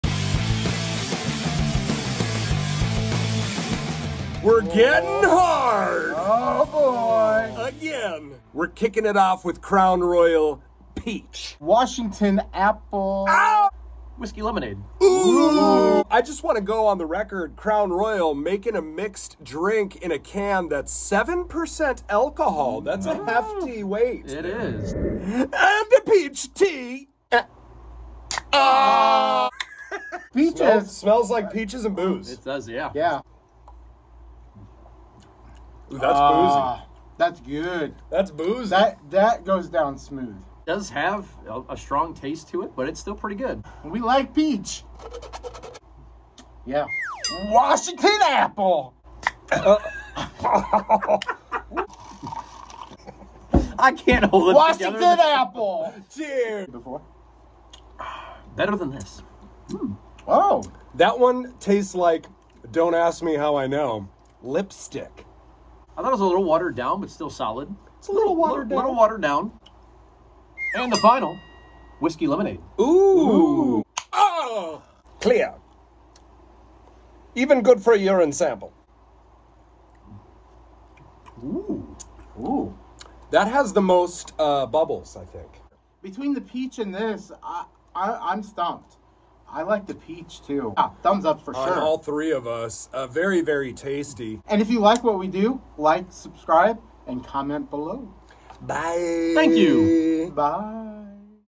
QLZ Acquired Tastes: Crown In A Can!
This week the guys try another alcoholic treat. Crown royal in a can. They were not disappointed. ￼